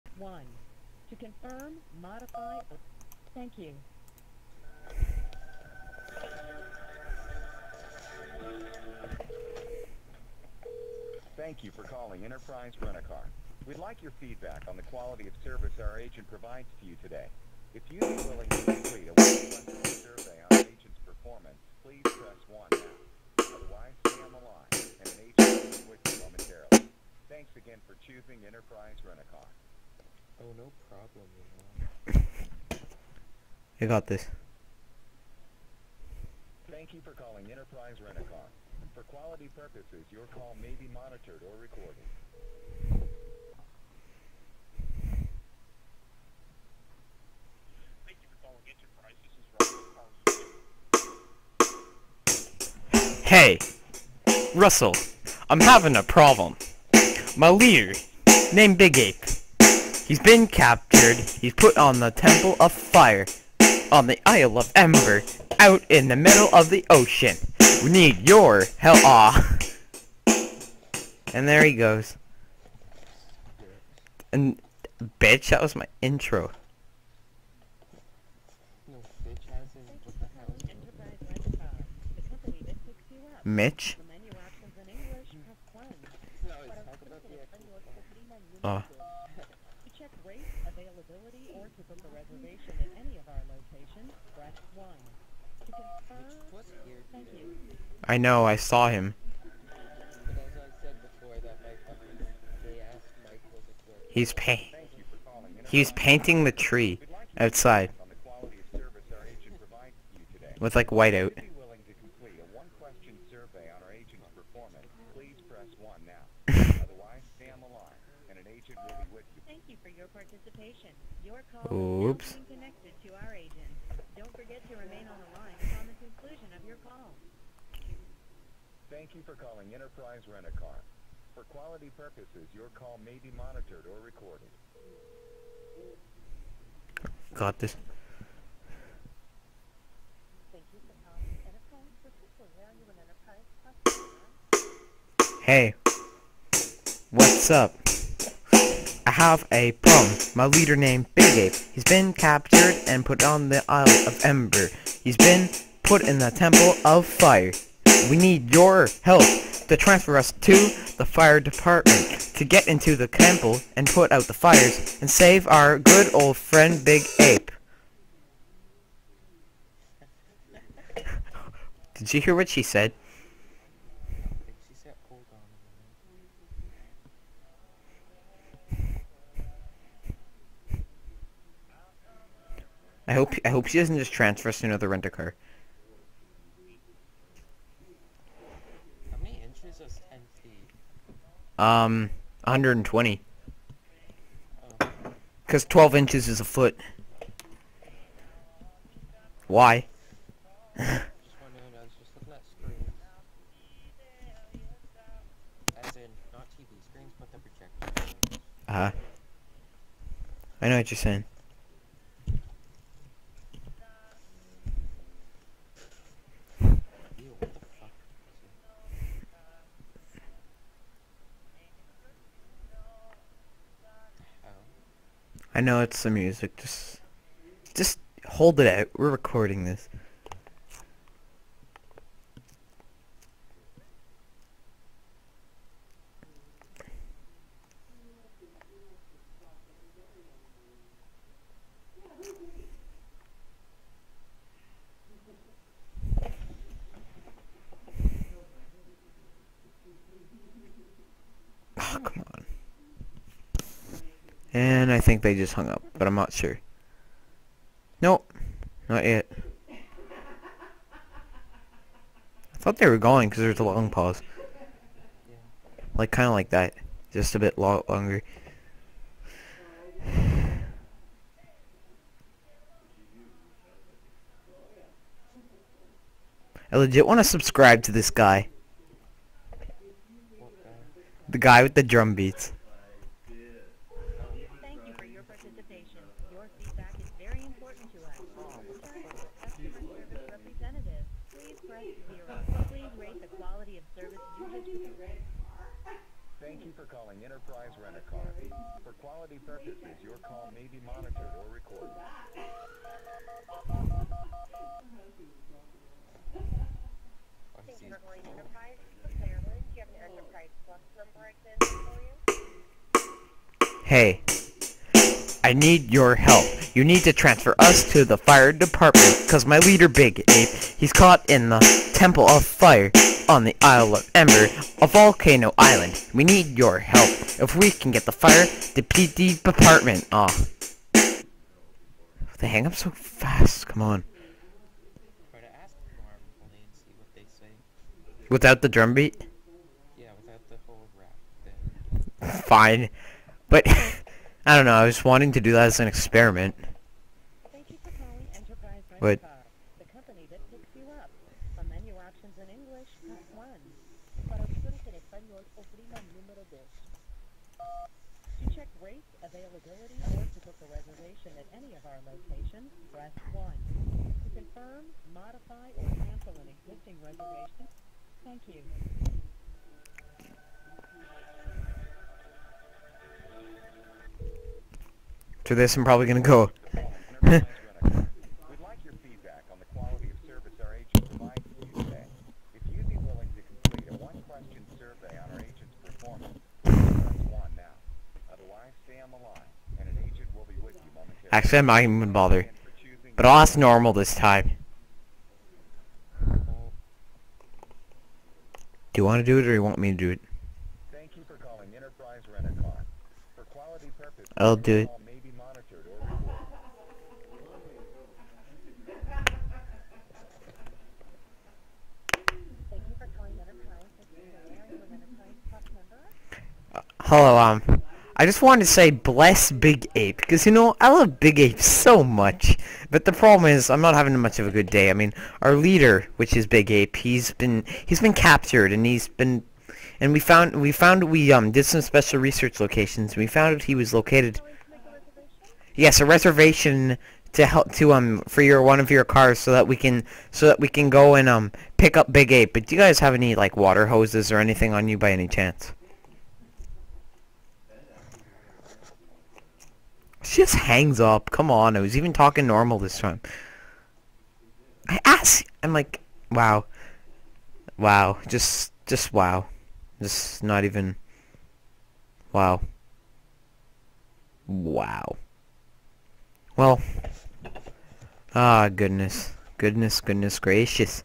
Enterprise Rent-a-Car Prank Call w/ Jonathan - Rap (2/2)
I decided to try another method of pranking this time. RAPPING!